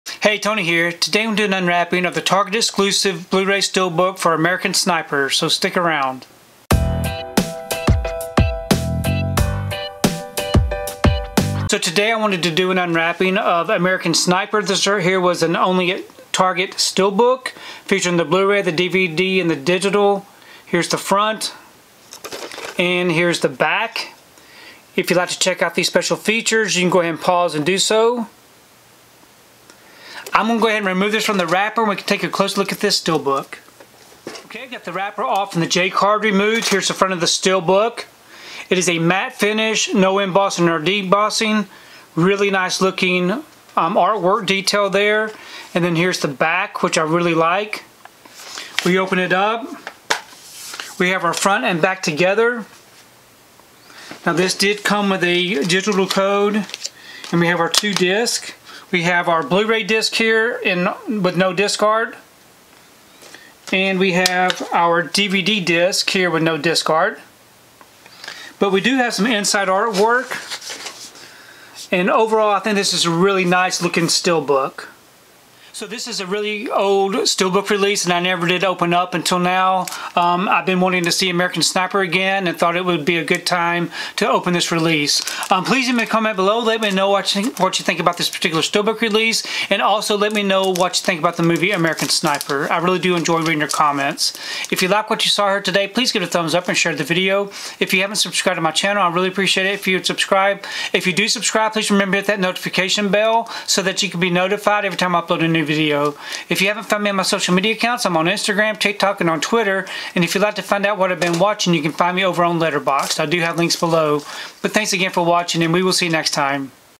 American Sniper Only at Target Blu-ray Steelbook
Check out the unwrapping of the American Sniper Only at Target Blu-ray Steelbook!

► Add the Blu-ray to your collection